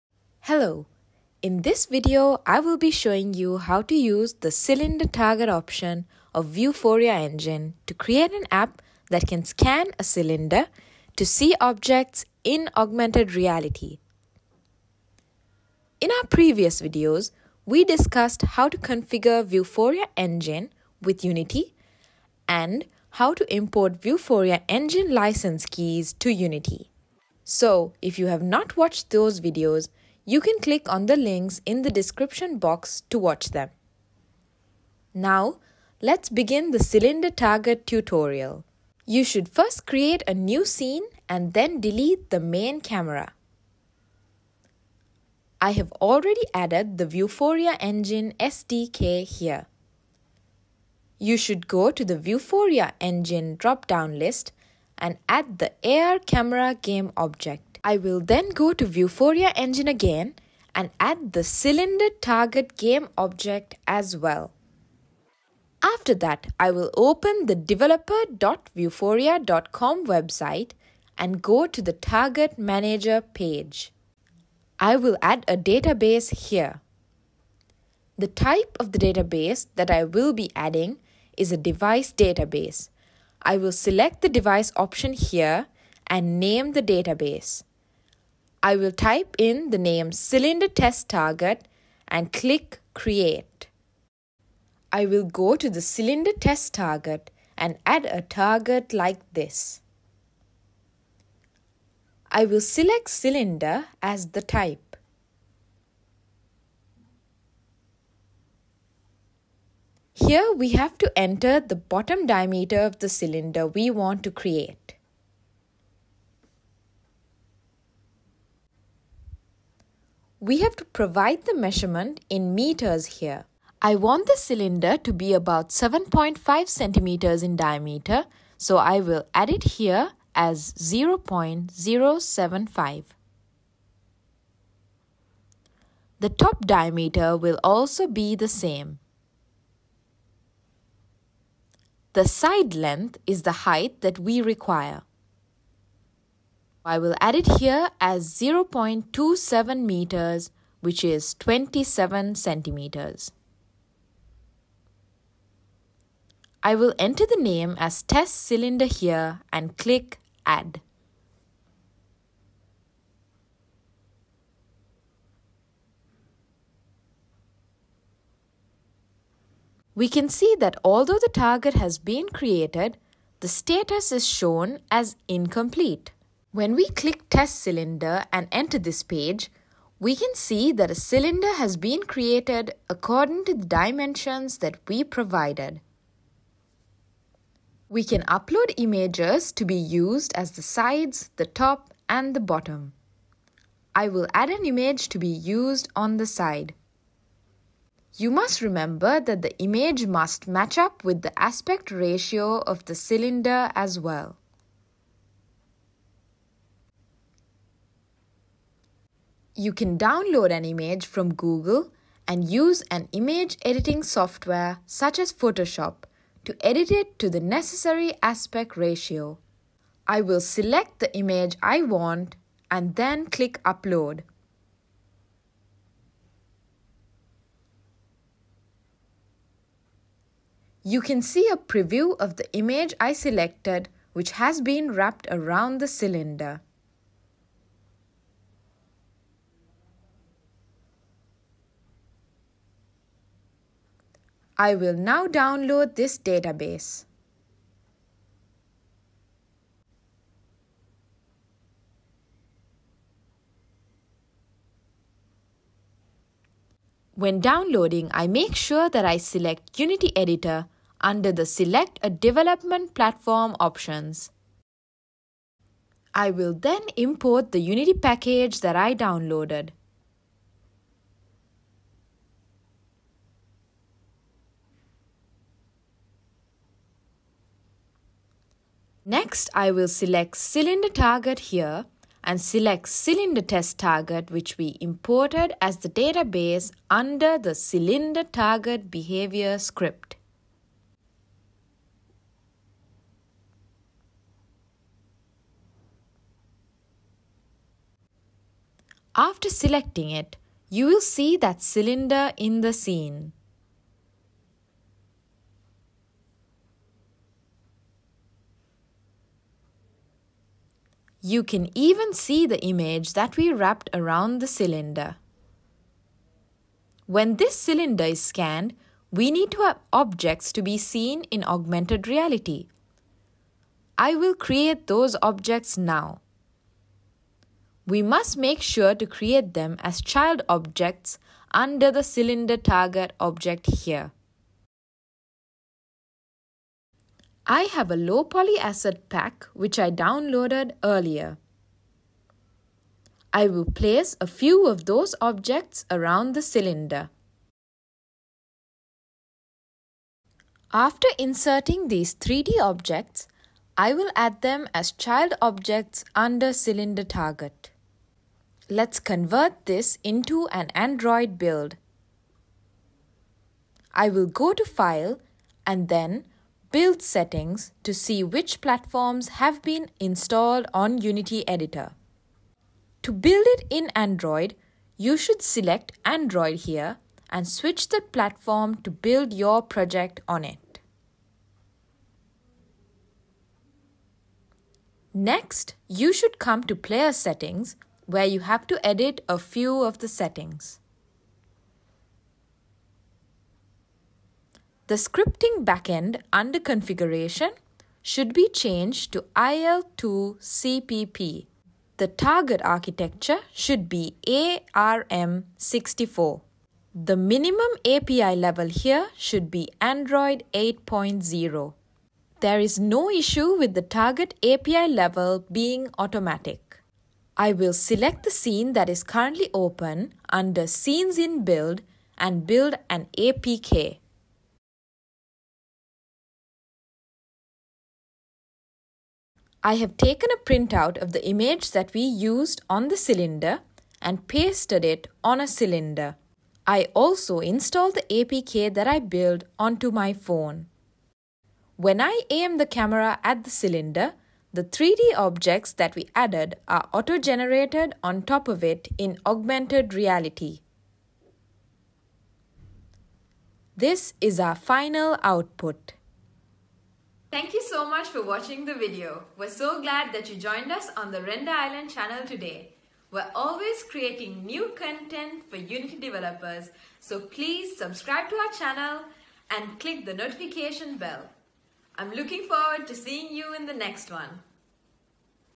Vuforia Unity Tutorial - Cylinder Targets #3
Learn the basics of Vuforia Cylinder Targets in this beginner-friendly tutorial! In this video, you will discover what Vuforia Cylinder Targets are and how to use them to create your own Augmented Reality experiences. Whether you are new to AR or a seasoned developer, this tutorial will give you a solid foundation for creating amazing AR experiences with Vuforia Cylinder Targets. So, join us and let's get started!

By the end of this tutorial, you will have a solid understanding of how to create and use Vuforia Cylinder Targets in your own AR projects. Whether you're a beginner or an experienced developer, this video will provide you with valuable insights and practical skills. So, let's get started!